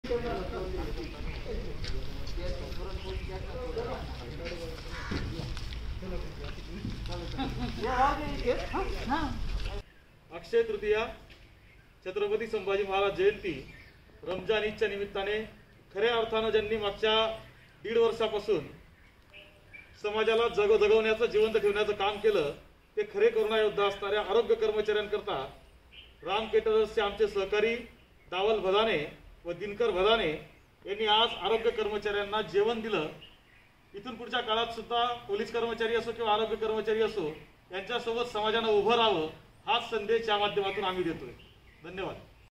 Ahmednagar | देवळा कोविड सेंटर आरोग्य कर्मचाऱ्यांना पुरण पोळी व आमरसाचे जेवण...
अहमदनगर - आज छत्रपती संभाजी महाराज जयंती, साडेतीन शुभ मुहूर्तांपैकी एक मुहूर्त अक्षय तृतीया, आणि मुस्लिम बांधवांचा अत्यंत पवित्र सण रमजान ईद या तिन्ही सणांची औचित्य साधून राम कॅटर्सच्या वतीने श्री डावल भदाणे व श्री दिनकर भदाणे या बंधूंनी देवळा येथील कोविड सेंटरच्या सर्व आरोग्य कर्मचाऱ्यांना पुरण पोळी व आमरसाचे जेवण दिले. याप्रसंगी किसान युवा क्रांती संघटनेचे अध्यक्ष श्री यशवंत गोसावी व त्यांचे सहकारी उपस्थित होते.